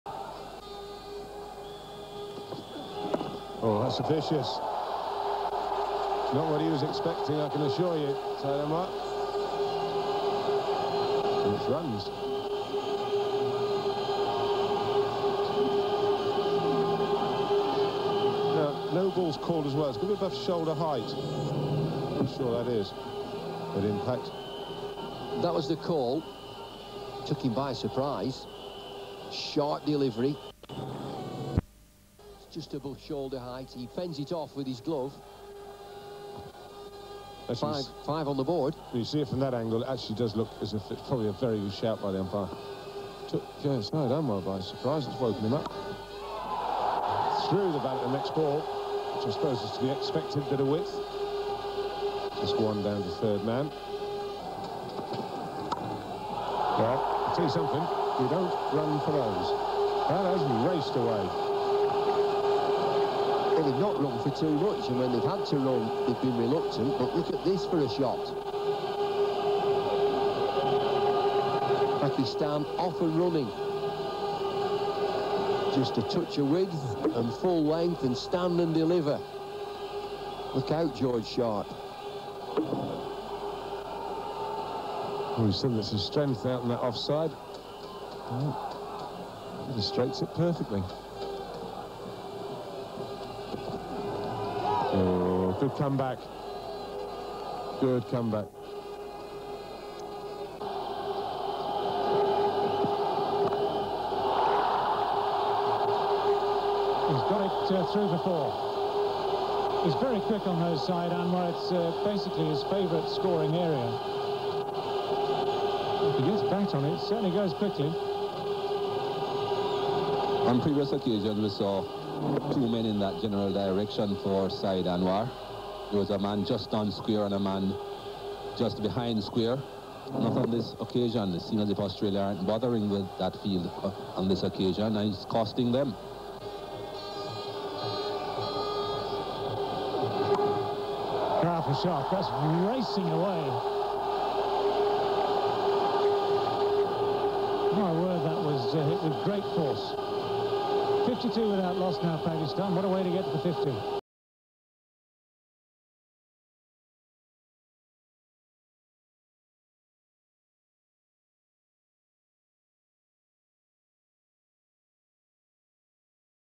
Saeed Anwar Facing Jason Gillespie l Surprised by Bouncer & then 2 Cracking Four l Trent Bridge 2001
Don't make Saeed Anwar ANGRY !
Brilliant Cricket !